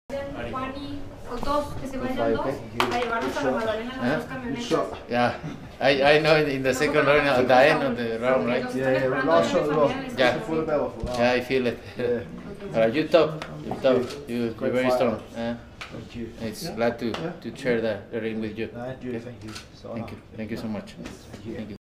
CANELO ALVAREZ & JOHN RYDER EMBRACE AFTER 12 ROUND WAR! ❤️
(Video courtesy of Matchroom)